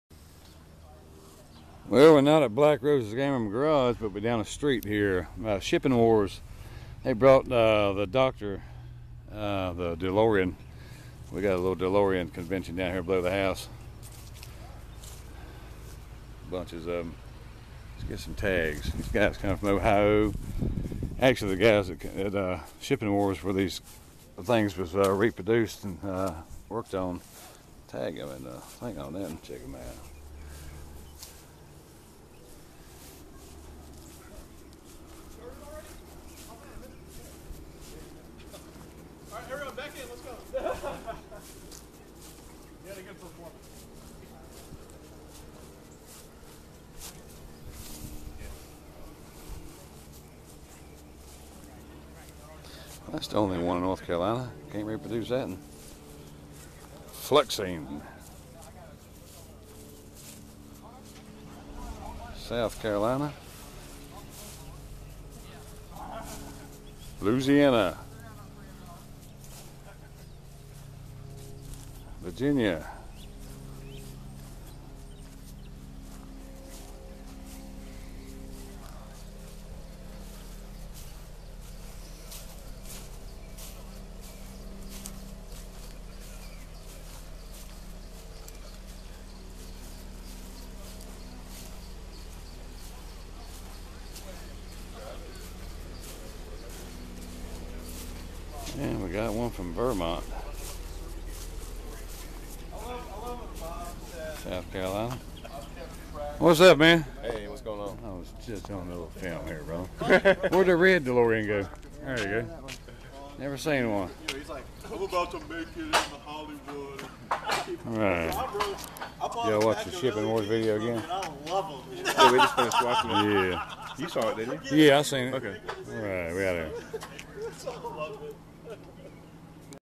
Delorean Tech- meet at Deckers N.Carolina
Had the Delorean Guys in Ohio from Shipping Wars at my Neighbors Home ,, Delorean's from the East Coast Club meet to have the Tech Guy's Install New Exhaust and Just give each a Through inspection ... No Flux Capacitors were Installed ...